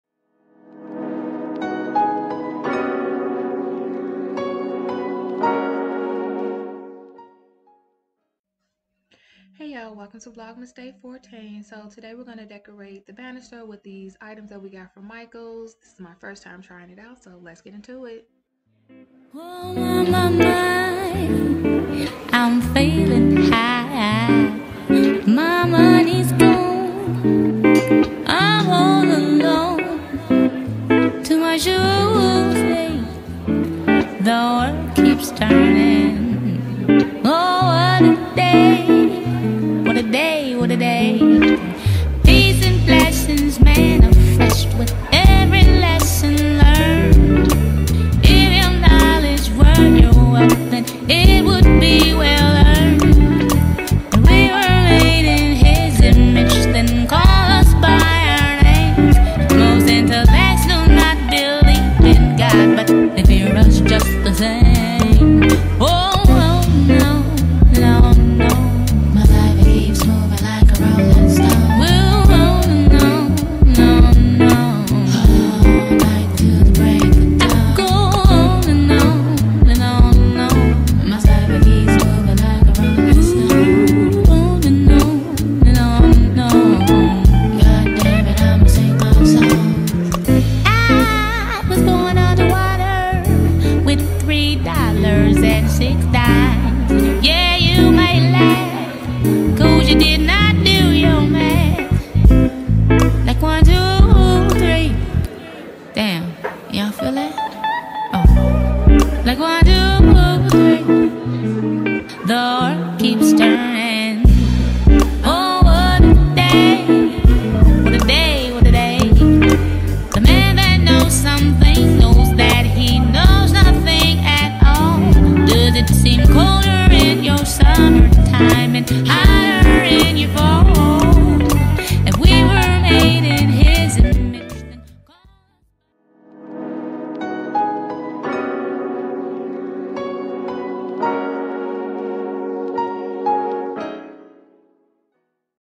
Holiday Banister Decorations, Vlogmas Day 14
Hey, y'all Welcome to Vlogmas 2022! In today's video, Holiday Banister Decorations, Vlogmas Day 14. I'll be decorating my home banister for the first time. All of the items used are from Michaels except for the ball decorations, they are from Five Below. This is one of my first DIY projects and my mom and I really enjoyed the process!

Thank y'all so much for tuning in for my first and definitely not last, Vlogmas journey, I have some nice things planned so y'all come back now...ya hear! lol.